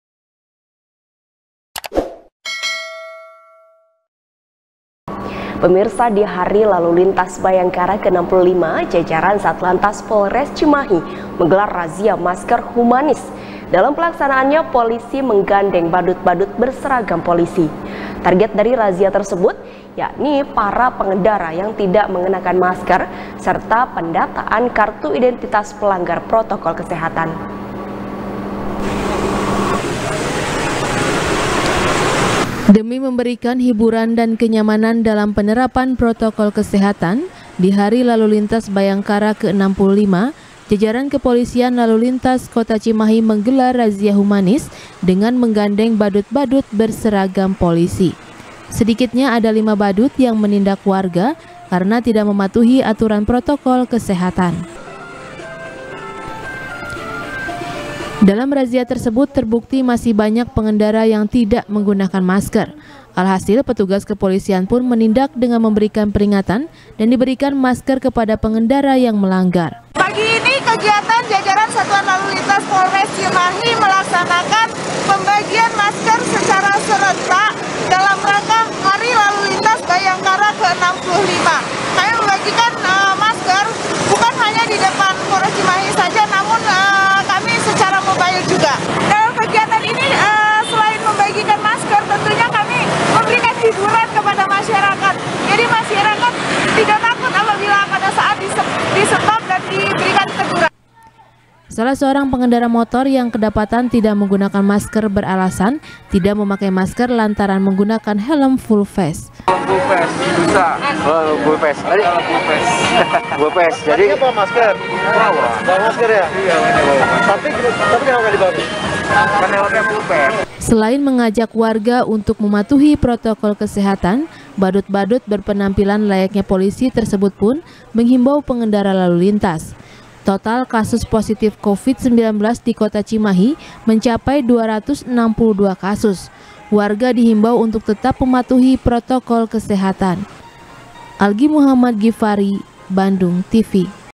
Razia Masker Humanis, Satlantas Gunakan Badut Berseragam Polisi | SBR | BANDUNGTV
SBR | BANDUNGTV | HANY SHANDRA | Di hari lalu lintas bayangkara ke 65, Jajaran Satlantas Polres Cimahi menggelar razia masker humanis. Dalam pelaksanaannya, polisi menggandeng badut-badut berseragam polisi. Target dari razia tersebut, yakni para pengendara yang tidak mengenakan masker, serta pendataan kartu identitas pelanggar protokol kesehatan.